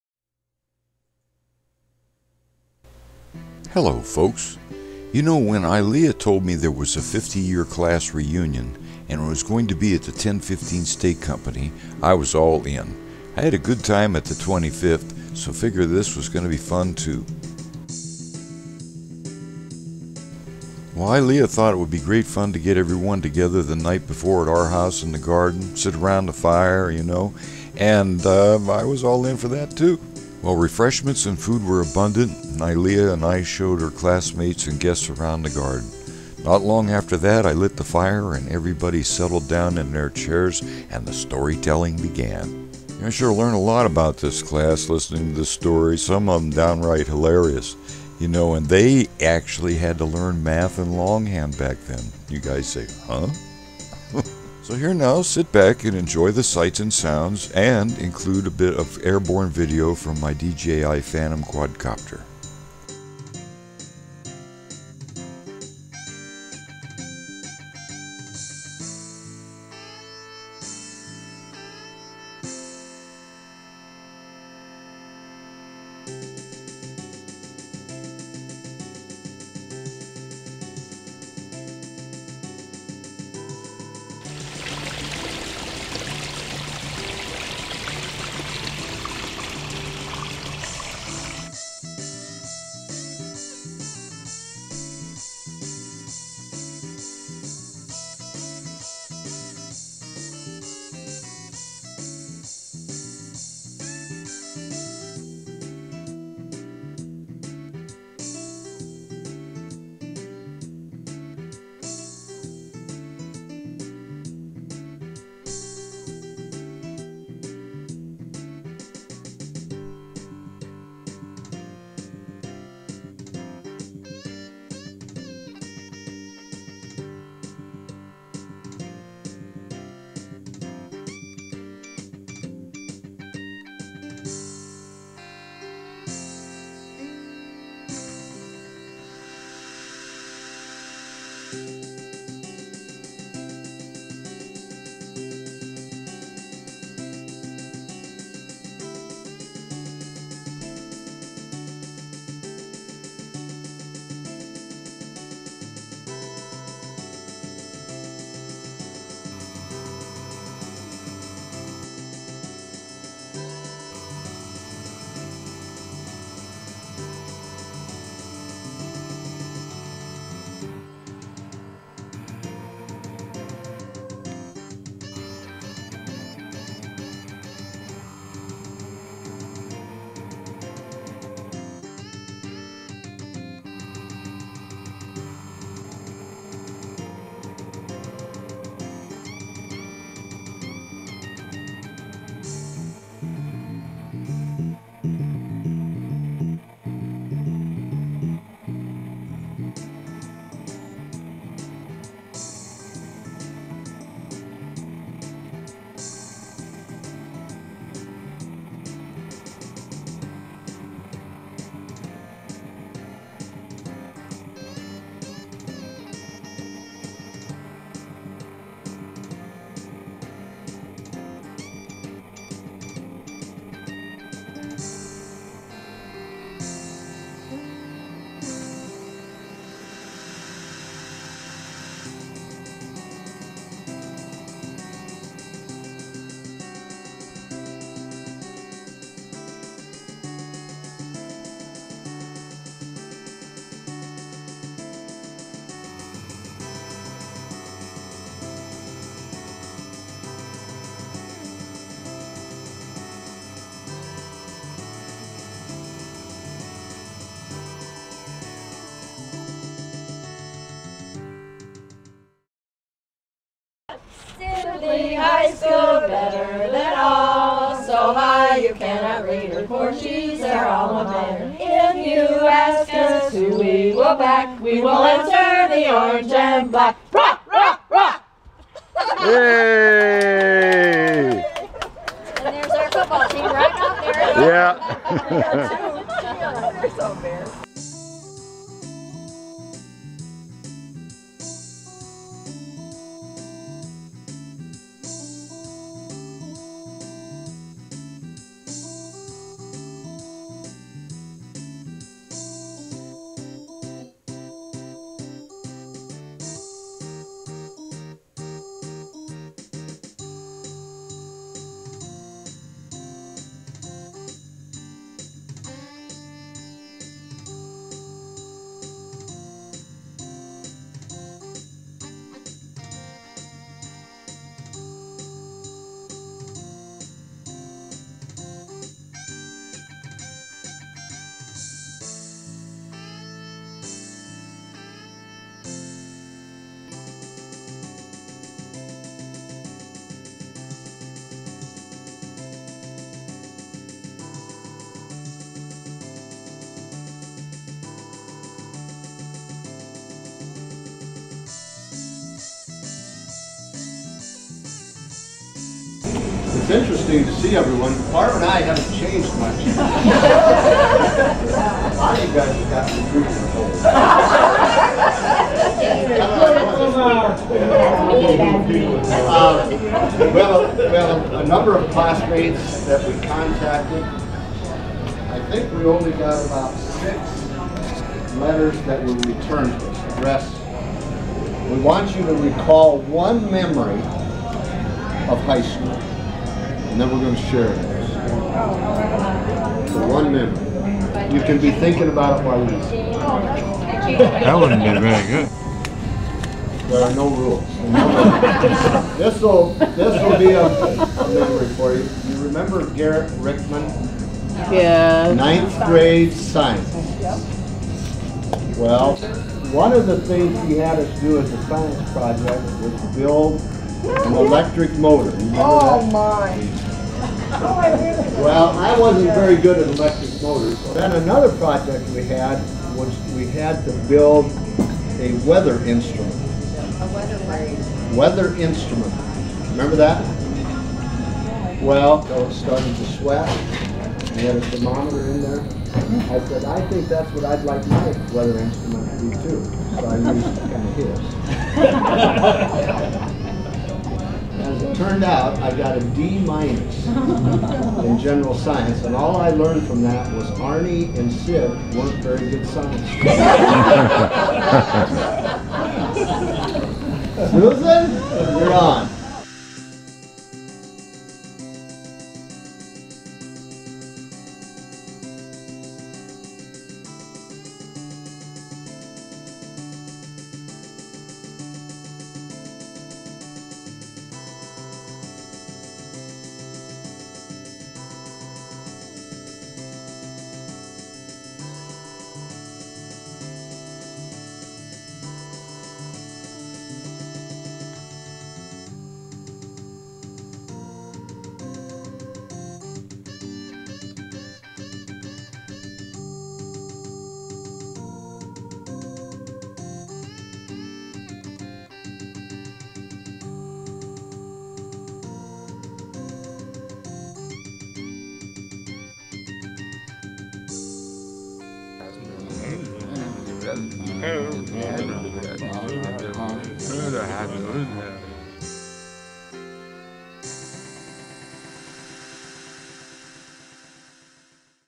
Sibley High School's 50th Reunion with Airborne Video too.
*Please Read. This video is mostly for those people and their friends and relatives that were Sibley High class of '63 graduates.  I used a DJi Phantom Quad for the Aerial pictures, of which I hope you enjoy, including the nostalgia of the old School days.
Special thanks to Ilea for the garden party and special thanks to all who participated.
Remember, "He who dares nothing, need not expect anything!"

Dave Herbert
AMA 8221
Certified Contest Director
Leader Member
Scientific Division.